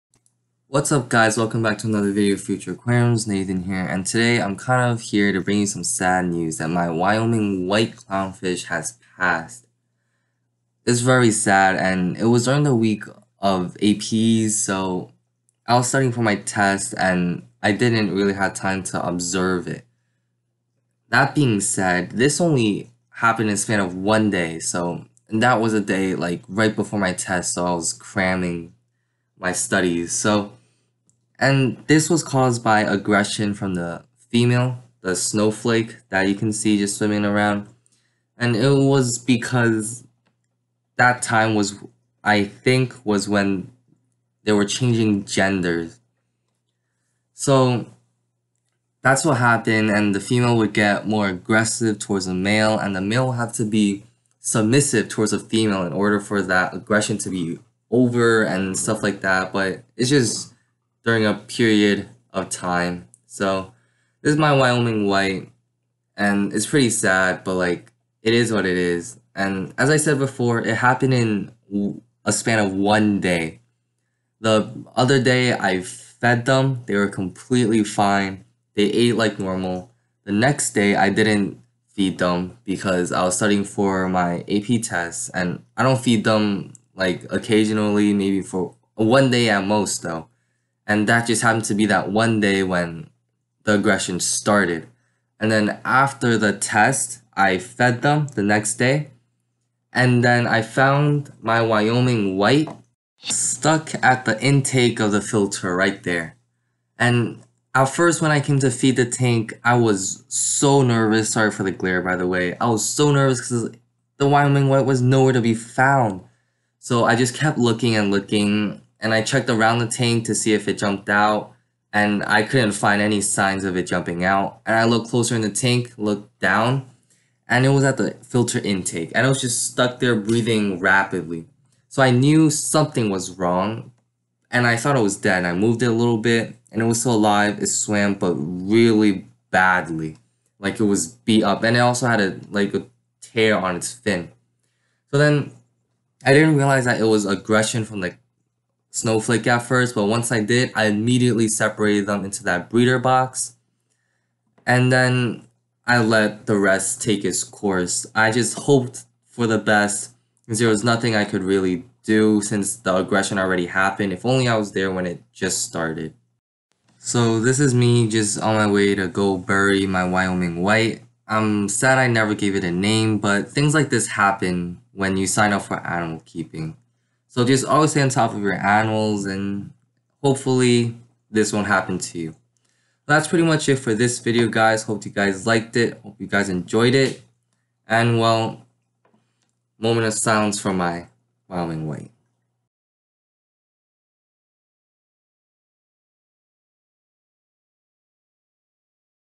Clownfish Aggression and My Wyoming white
Clownfish aggression is something you always have to look out for. Especially when you buy juveniles. BE CAREFUL AND DON"T MAKE THE MISTAKE I DID!!! I will make a video soon on how to avoid the aggression but until then, stay on top of your fish!!